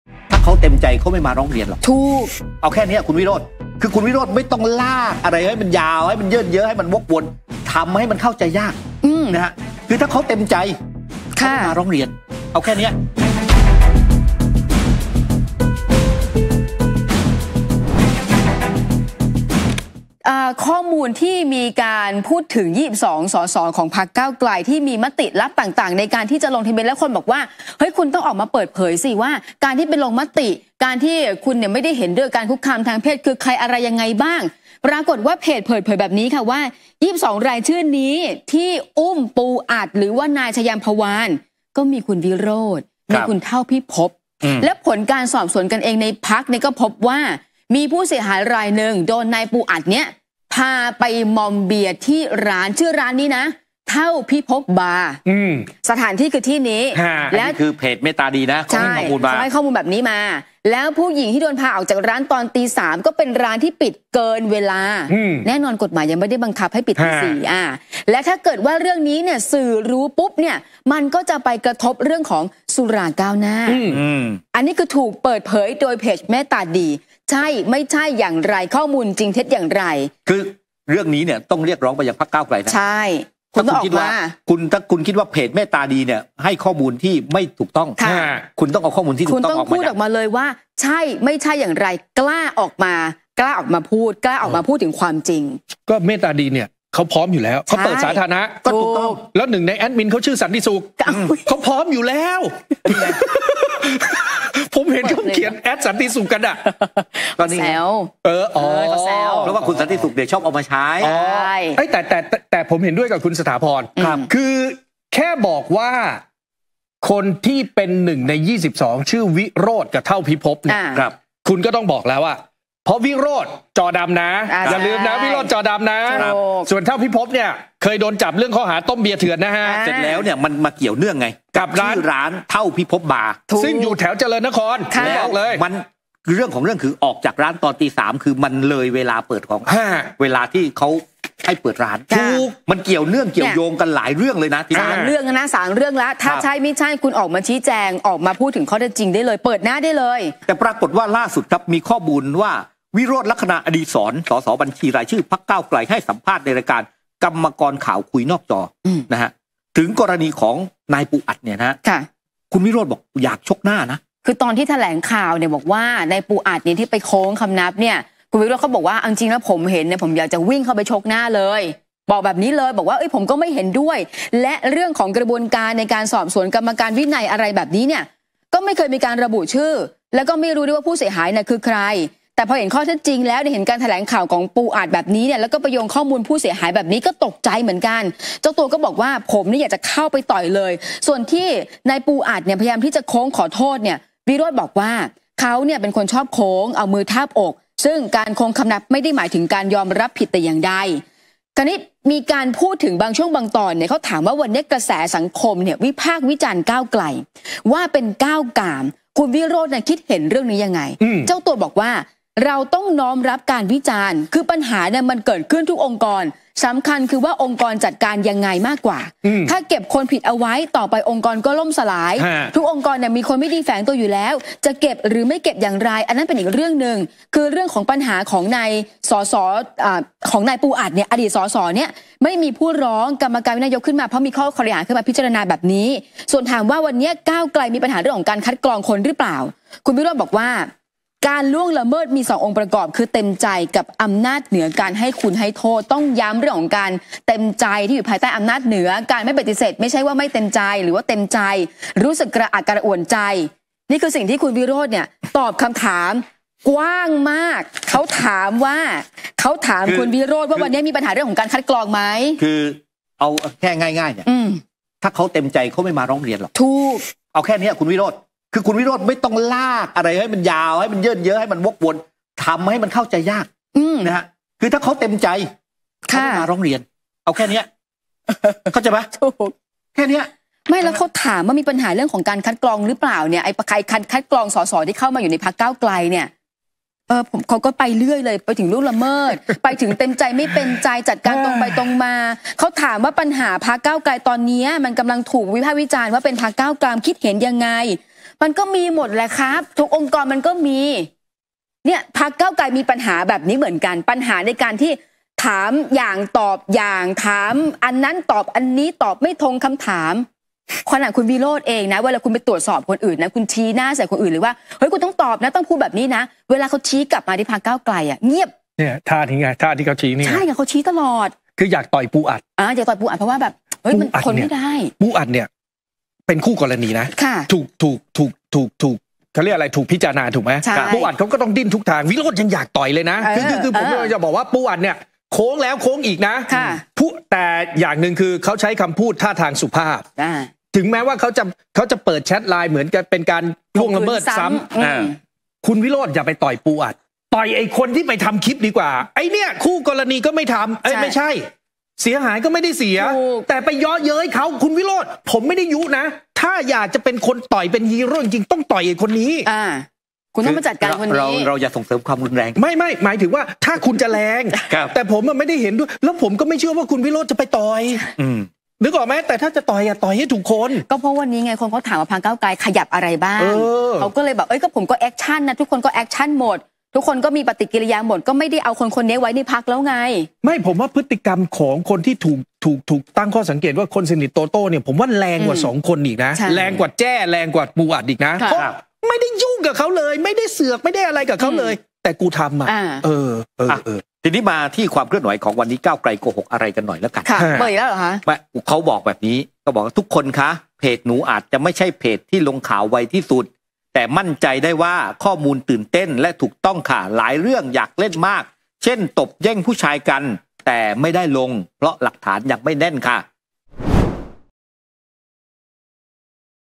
เพจดัง เปิด "มติลับพรรคก้าวไกล" โหวตไม่ขับ "ปูอัด" 1 ใน 22 มี "วิโรจน์" อยู่ด้วย | TOPNEWS ONLINE
เพจ The METTAD เปิด "มติลับพรรคก้าวไกล" โหวตไม่ขับ "ปูอัด" 1 ใน 22 เสียงนั้น มี "วิโรจน์" รวมอยู่ด้วย

ติดตามข่าวสารจากทางเราได้ทางทีวีดิจิตอล ช่อง JKN18